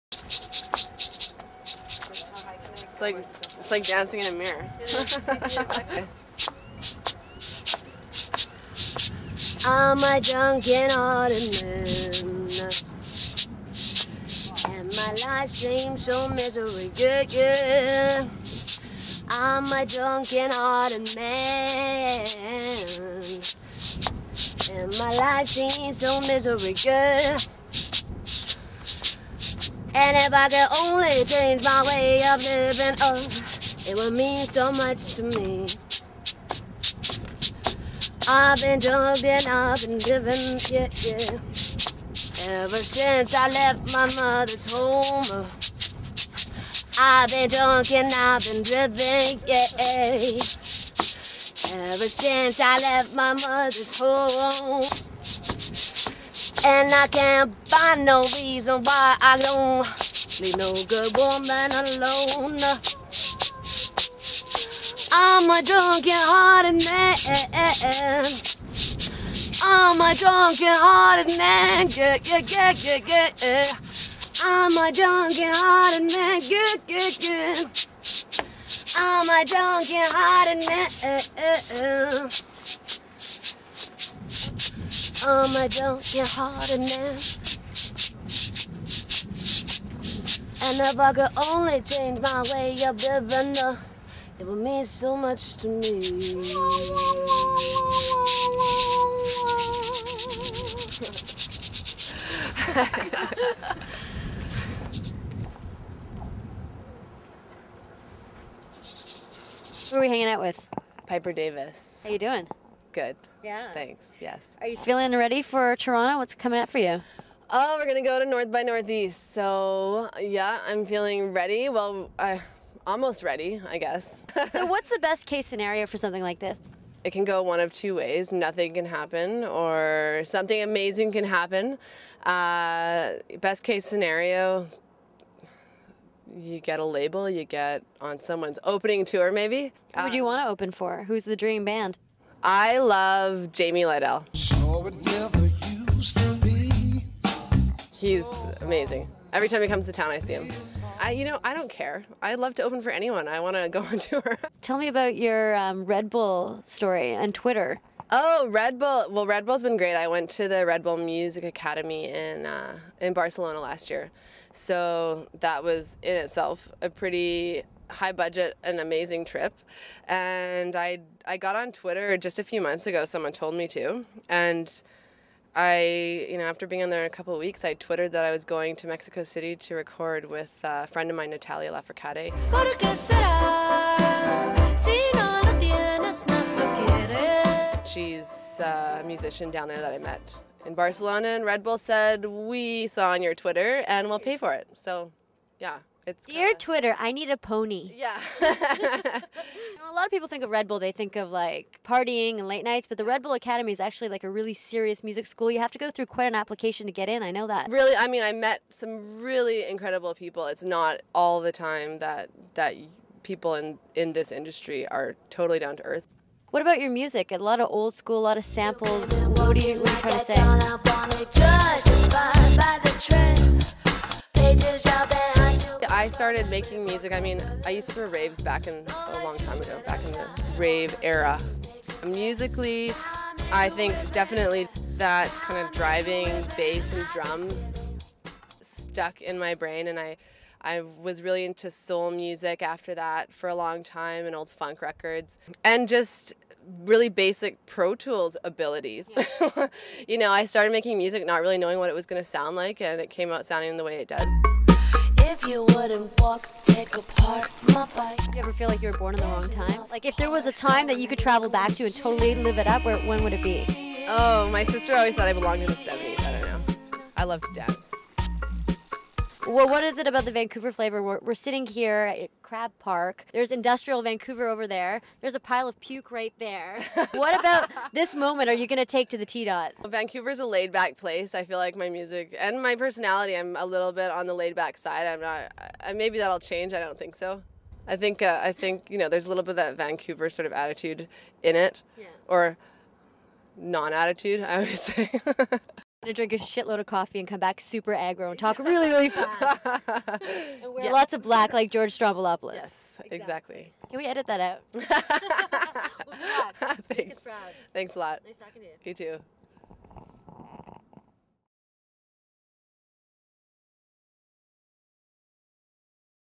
winnie tv episode 4 - Piper Davis
winniecooper.net presents a take away show with Piper Davis and interview.  Shot in Vancouver BC, 2009